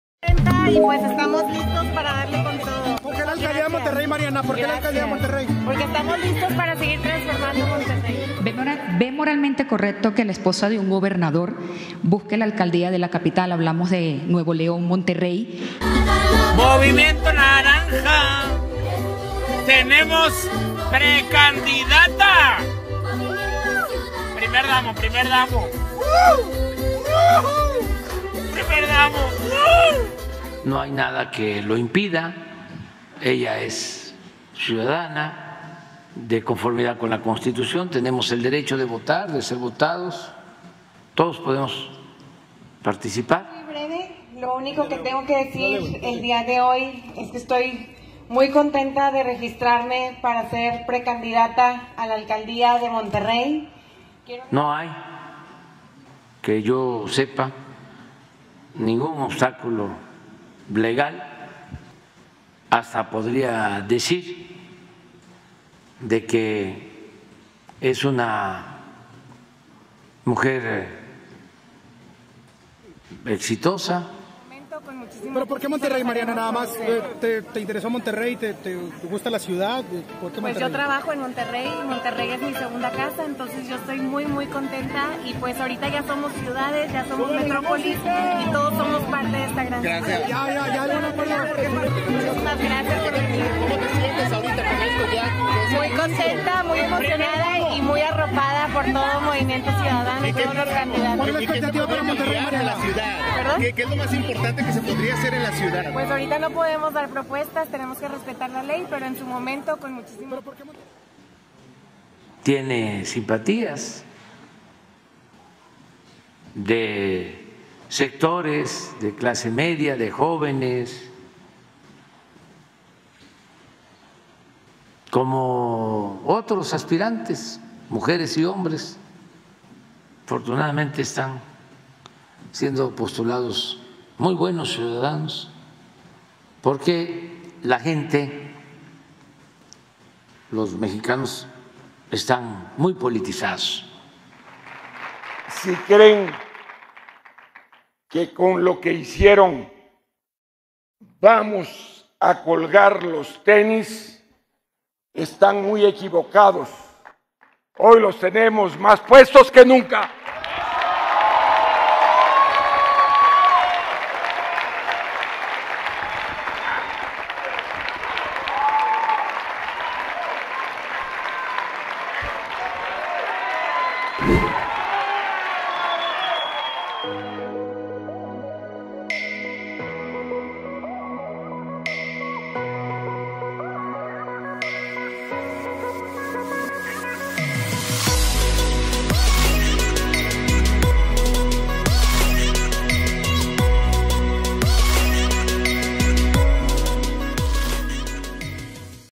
Esto dijo AMLO sobre el destape de Mariana Rodríguez.
Samuel García no cuelga los tenis Fosfo-Fosfo. Destapa a Mariana Rodríguez para la alcadía de Monterrey y ya se siente "Primer Damo". Esta fue la respuesta de AMLO al ser cuestionado en la mañanera sobre el destape de los Fosfo-Fosfo.

La prensa se regula con la prensa.